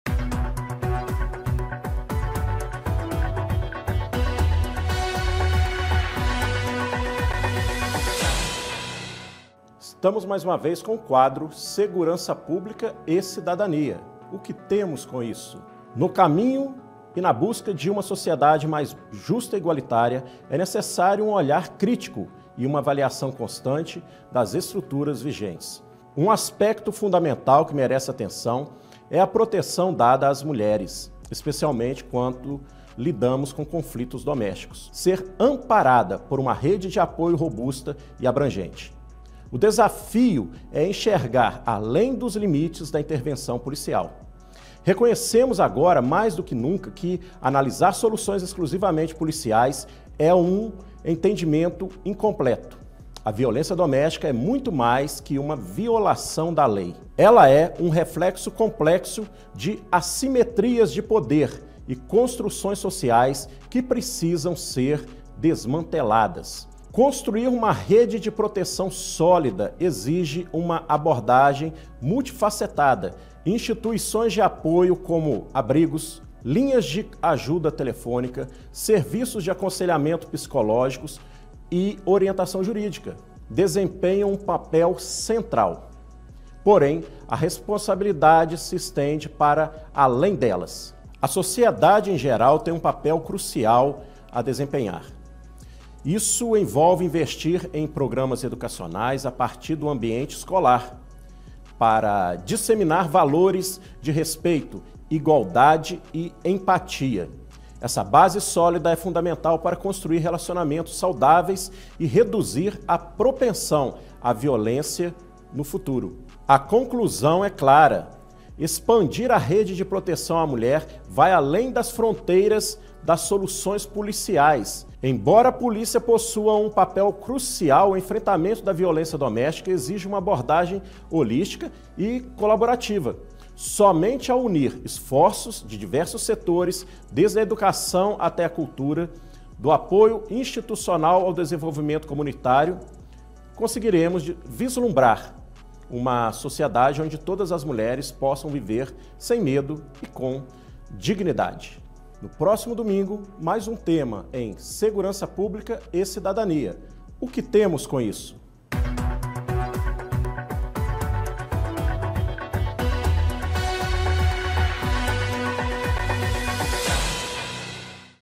Segurança Pública e Cidadania: Ampliando a Rede de Proteção à Mulher - Além das Soluções Policiais
No caminho na busca de uma sociedade mais justa e igualitária, é necessário um olhar crítico e uma reavaliação constante das estruturas vigentes. Um aspecto fundamental que merece atenção é a proteção às mulheres, especialmente quando lidamos com conflitos domésticos. Romper com paradigmas exige compreender que a segurança das mulheres não pode ser tratada exclusivamente nas mãos da polícia, mas sim ser amparada por uma rede de apoio robusta e abrangente.